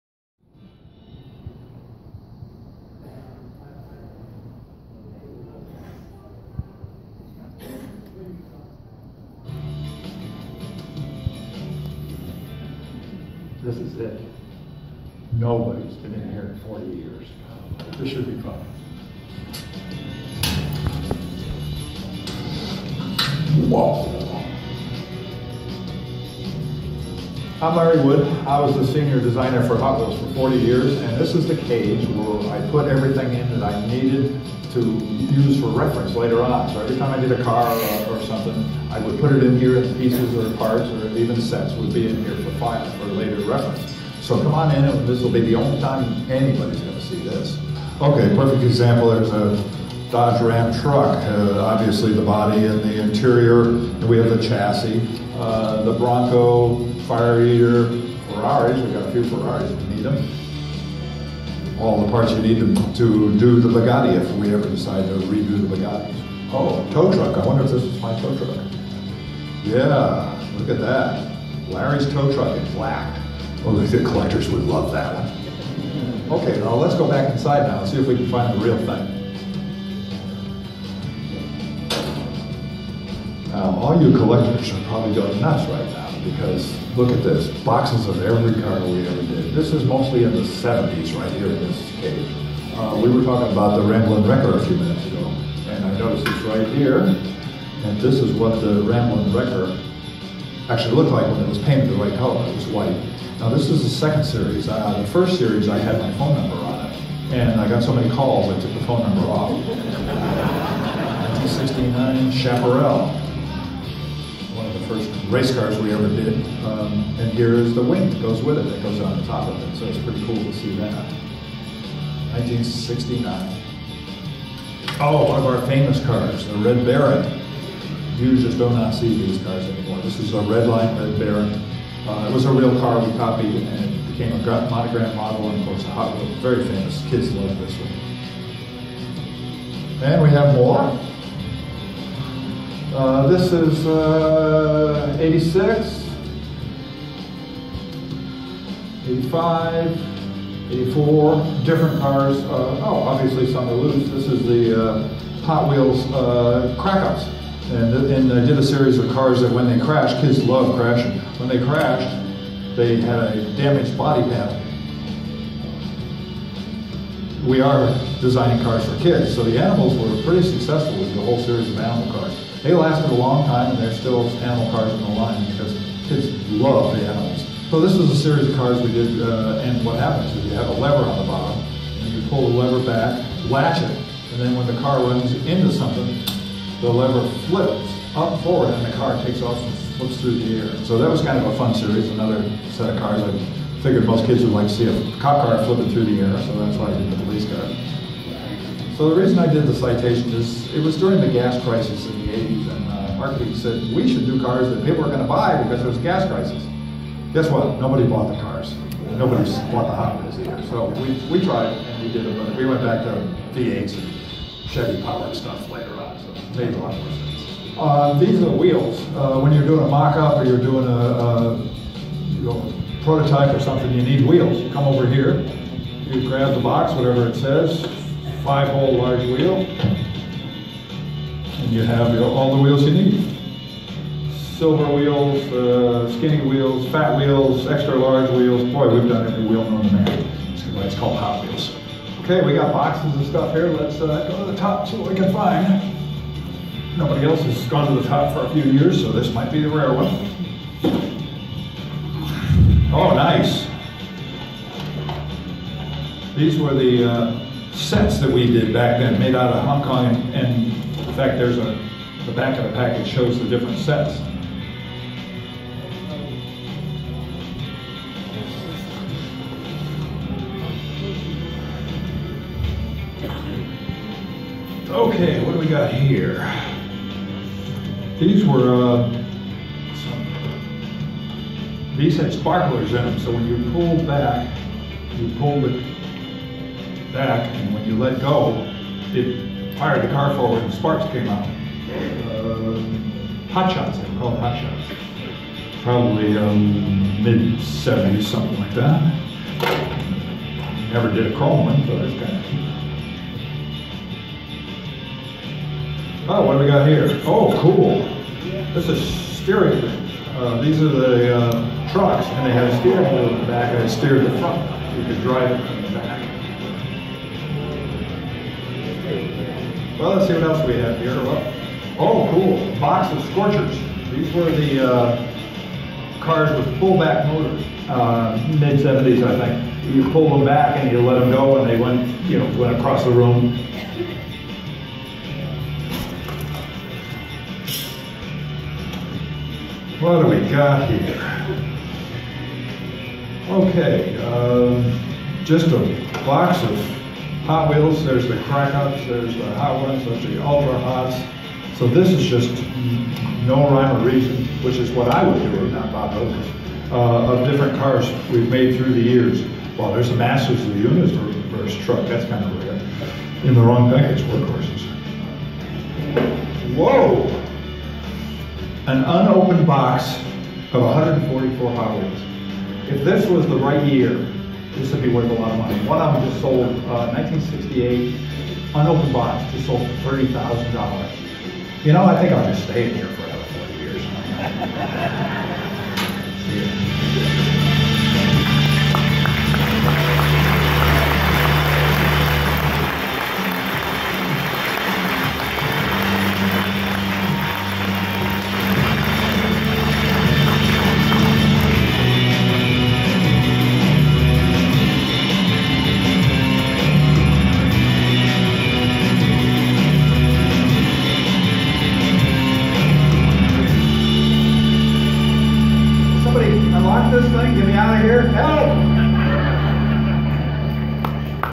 Larry Wood Hot Wheels Vault
Larry Wood going through old Hot Wheels in the Mattel vault!